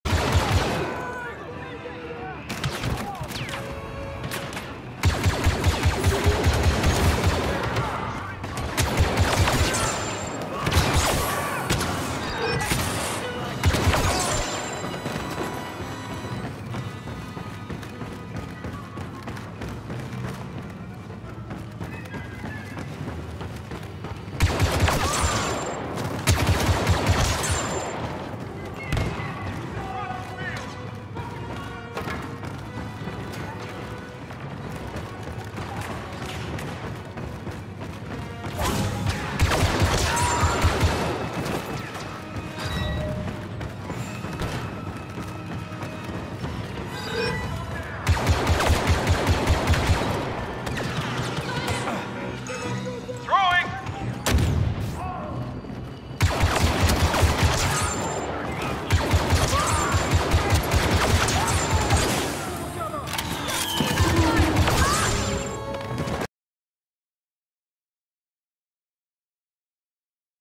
Stormtroopers Invade Cloud City!
Imperial Stormtroopers have invaded Cloud City on Bespin!